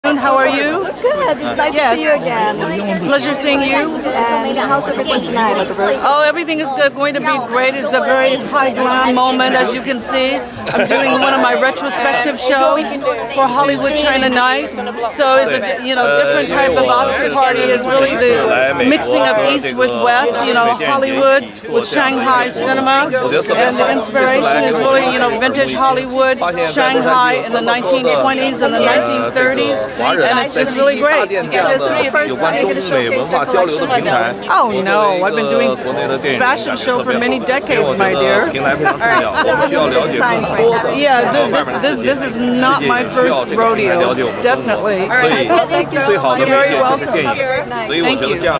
INTERVIEW WITH SUE WONG ACCS "HOLLYWOOD CHINA NIGHT AWARD CEREMONY" WITH OSCAR VIEWING DINNER 2019
*Interview with Fashion Designer, Sue Wong at the Red Carpet*
*Photos & Videos By: PaparazzCEL Celia Abuel @paparazzcel Celia Sabino Abuel @celabuel*
*Interview & Live Streaming By: @amazingrace20 Grace Aguilar*
*ACCS {American-Chinese CEO Society} Presents: "HOLLYWOOD CHINA NIGHT AWARD CEREMONY' With OSCAR VIEWING Dinner  Held at the Prestigious Taglyan in Hollywood*
*Along With Special Guests: Fashion Designer Sue Wong, Actors/Producers Billy Zane {Titanic, Back to the Future}, Vincent de Paul {Poseidon, Beverly Hills Christmas}, Christopher Lambert {Beowolf}, Basil Moore {The C Gate}, etc. more to post such as Meta World Peace {NBA Player/Coach}, Sean Oliver {JFK, Nixon, The Door, Savages}, Larry Kasanoff {Terminator, True Lies, Mortal Kombat} ,Lisa Lu {Crazy Rich Asians, Joyluck Club}, Larry Namer {E! Entertainment Founder}, Mick Davis {Modigliani, Red Lotu}, Christine Divine {Independence Day/Newscaster}, Sean McNabb {Rock Story}, Robert Frank Teller {Mr. Deeds}, Larry Dunn {Musician}, Basil Moore {Filmmaker/Director}, Robert Sun {CEO ACCS}, Gray Sze Kong {President & Founder of The 1 Wine and Classic New York Beer}, Billie Mitchell {CEO, The 1 Wine and Classic New York Beer} *